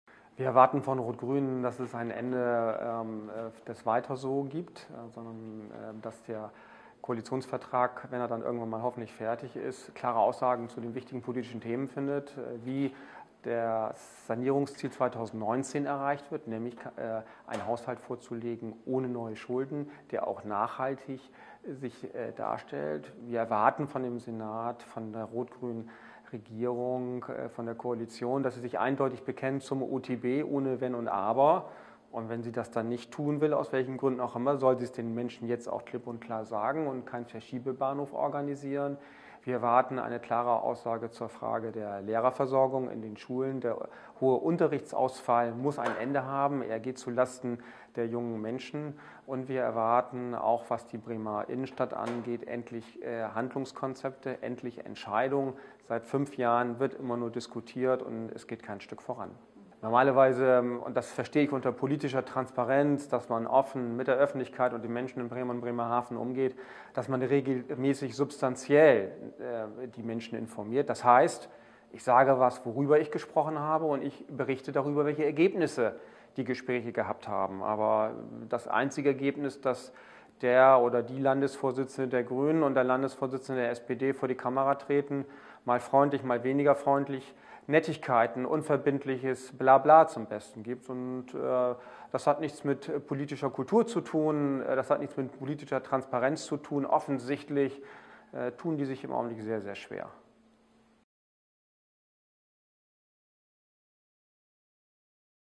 Kastendiek: "Unverbindliches Blabla"
Die bisherigen Ergebnisse und Vereinbarungen der Koalitionsverhandlungen zwischen SPD und Grünen sind dürftig und unkonkret und unverbindliches Blabla. Der CDU-Landesvorsitzende Jörg Kastendiek hat heute gemeinsam mit dem Fraktionsvorsitzenden Thomas Röwekamp in einer Pressekonferenz ein Positionspapier vorgestellt, das aus Sicht der Christdemokraten die essentiellen Anforderungen für eine Regierungspolitik der kommenden vier Jahre umfasst. Bei dem Thema Finanzen fordert die CDU ein Volksentscheid über die Haushaltssanierung. Andere wichtigeThemen im Positionspapier sind Bildung, Wirtschaft, Stadtentwicklung, Innere Sicherheit und Sozialpolitik.